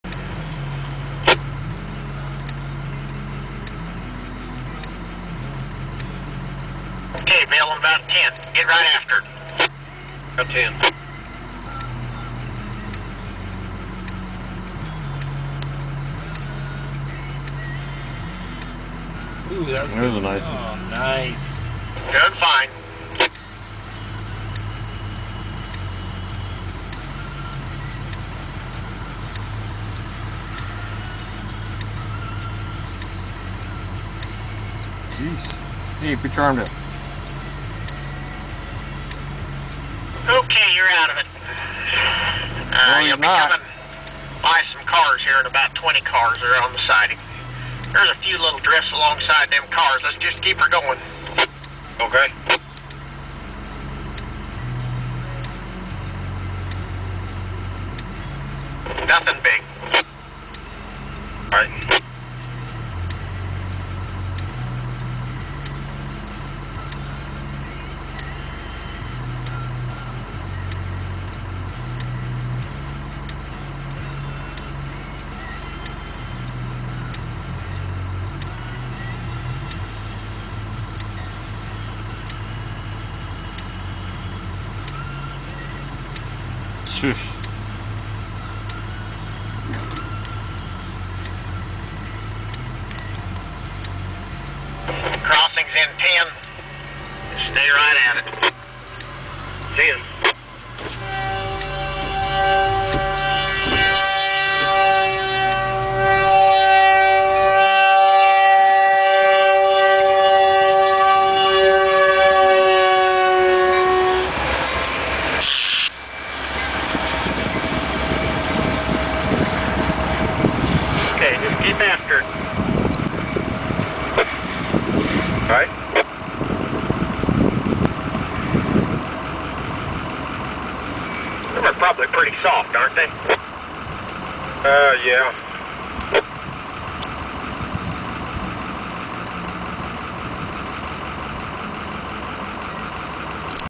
Train Snow Plowing 2
Plowing snow at Goodland Ks. on the Kyle Railroad.